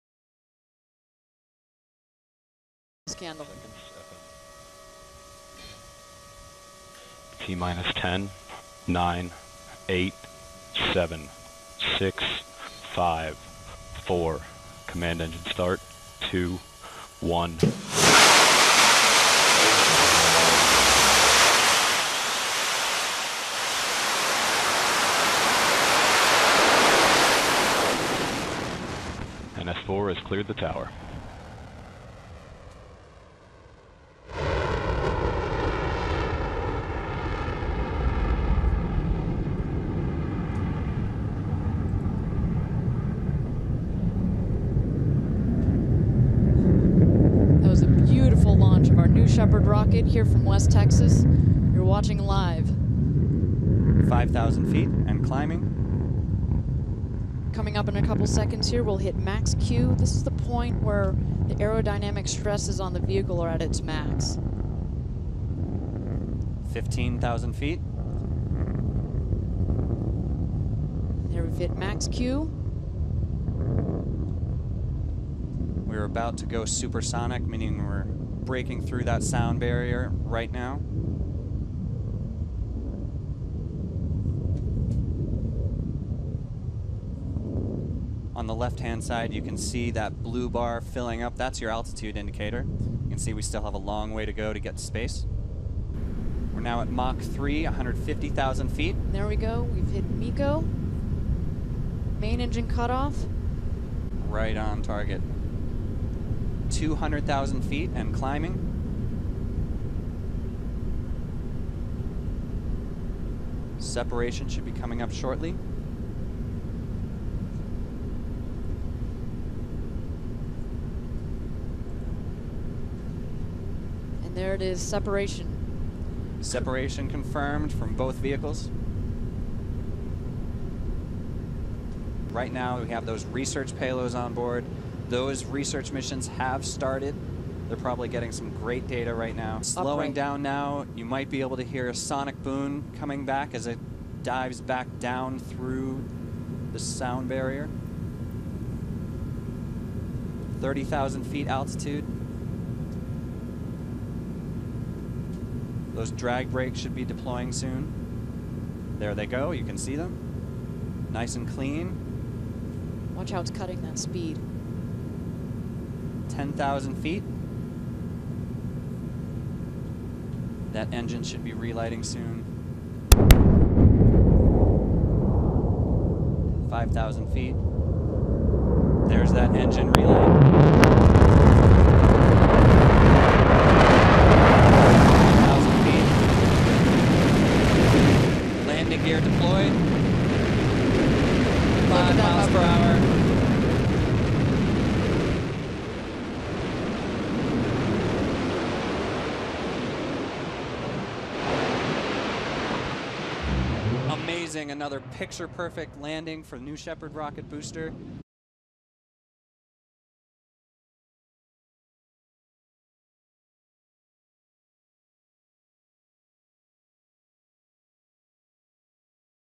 New Shepard landing, 19 June 2016
The same Blue Origin's New Shepard space vehicle underwent the fourth test mission from the West Texas Suborbital Launch Site, on 19 June 2016. The rocket launched at 14:35 UTC (10:35 ET) with a payload from three universities.

Credit:
Blue Origin